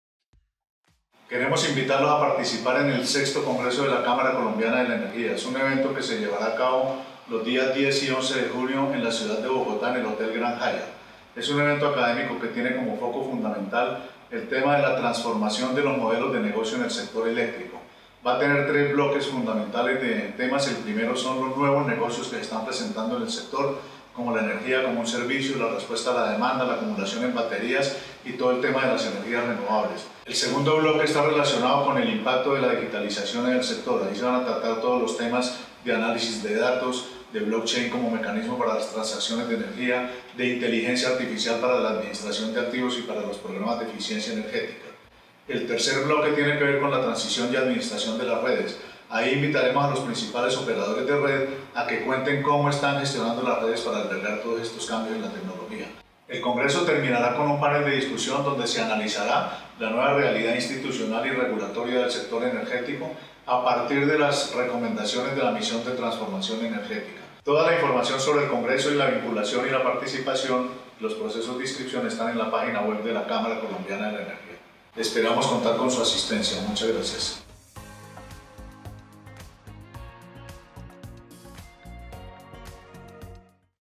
VI Congreso CCENERGÍA "La Transformación de los Modelos de Negocio en el Sector Eléctrico"
OBJETIVO
Presentar, analizar y compartir con los empresarios del sector los nuevos modelos de negocio que se están desarrollando como resultado de la transición energética. Es importante estar actualizado para aprovechar los nuevos espacios de negocios y mejorar la competitividad de las empresas.

DIRIGIDO A
El VI Congreso de la Cámara Colombiana de la Energía – CCEnergía, está dirigido a: entidades del gobierno, gremios del sector, directivos de empresas del sector, afiliados a la Cámara Colombiana de la Energía, Academia, consultores y contratistas independientes